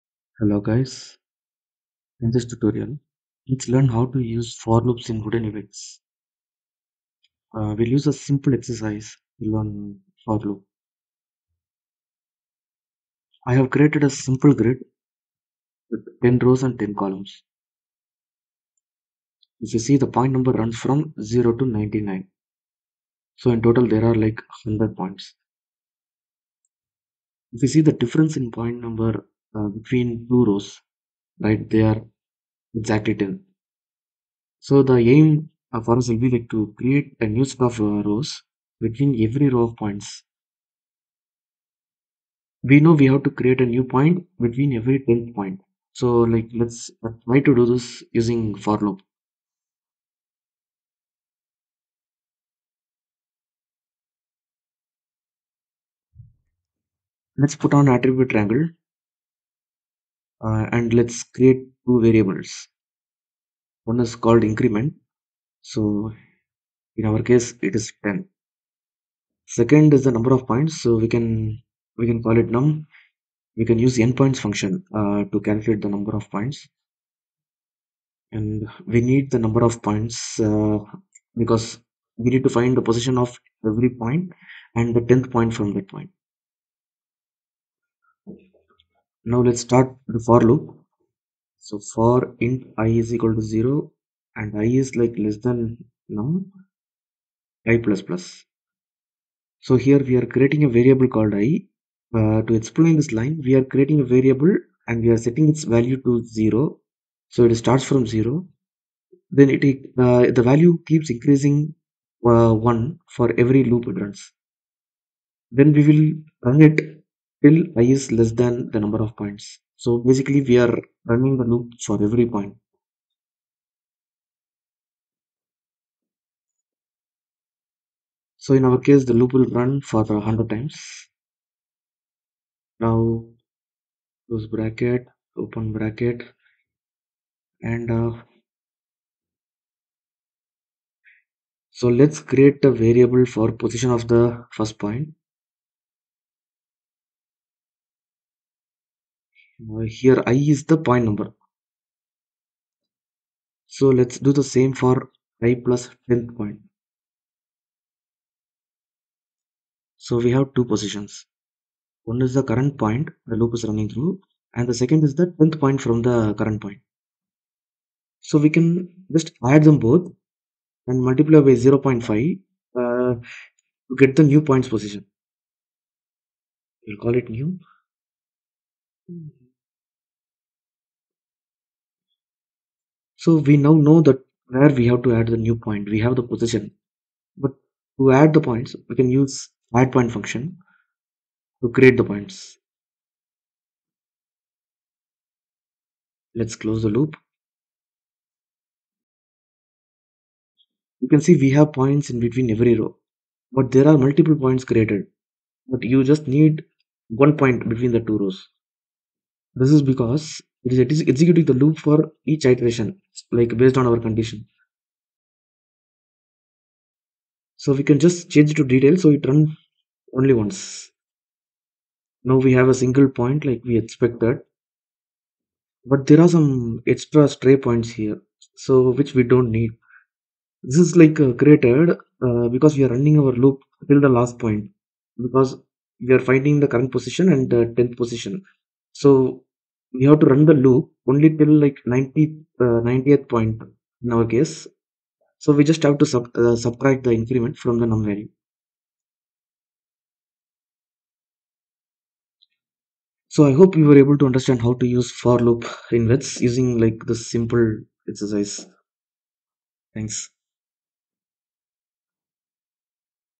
For Loops in VEX: Houdini Tutorial - Beginners
hello,

You can download the hip file from here for free

We will take a look at "For Loop" in Vex using Houdini. We will solve a simple problem and try to learn "For Loop" using it.